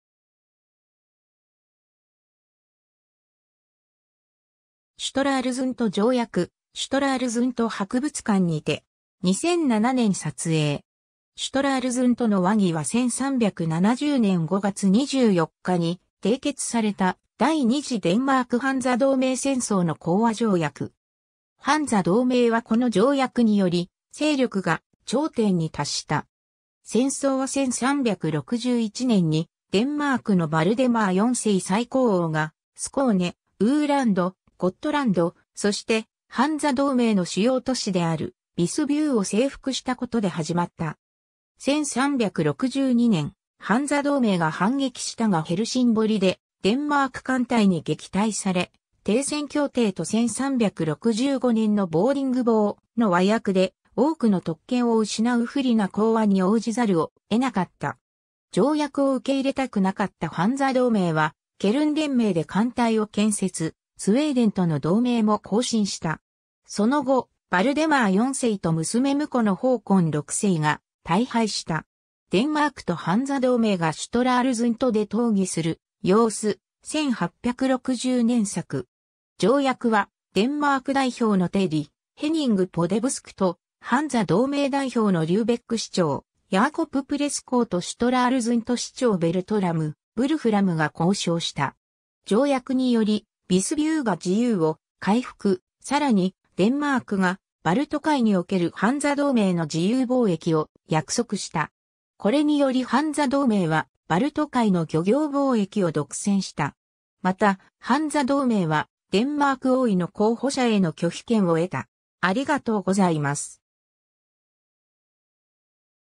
シュトラルズントの和議 (1370年)
アルブレクト_(スウェーデン王)
シュトラールズント条約、シュトラールズント博物館にて。
2007年撮影。
 シュトラールズントの和議（シュトラールズントのわぎ、ドイツ語: Friede von Stralsund）は1370年5月24日に締結された、第二次デンマーク・ハンザ同盟戦争の講和条約。
ハンザ同盟はこの条約により勢力が頂点に達した。
 戦争は1361年にデンマークのヴァルデマー4世再興王がスコーネ、ウーランド、ゴットランド、そしてハンザ同盟の主要都市であるヴィスビューを征服したことで始まった。
1362年、ハンザ同盟が反撃したがヘルシンボリでデンマーク艦隊に撃退され、停戦協定と1365年のヴォーディングボーの和約で多くの特権を失う不利な講和に応じざるを得なかった。
条約を受け入れたくなかったハンザ同盟はケルン連盟で艦隊を建設、スウェーデンとの同盟も更新した。
その後、ヴァルデマー4世と娘婿のホーコン6世が大敗した。
 デンマークとハンザ同盟がシュトラールズントで討議する様子、1860年作。
 条約はデンマーク代表の廷吏ヘニング・ポデブスクとハンザ同盟代表のリューベック市長ヤーコプ・プレスコウとシュトラールズント市長ベルトラム・ヴルフラムが交渉した。
条約によりヴィスビューが自由を回復、さらにデンマークがバルト海におけるハンザ同盟の自由貿易を約束した。
これによりハンザ同盟はバルト海の漁業貿易を独占した。
また、ハンザ同盟はデンマーク王位の候補者への拒否権を得た。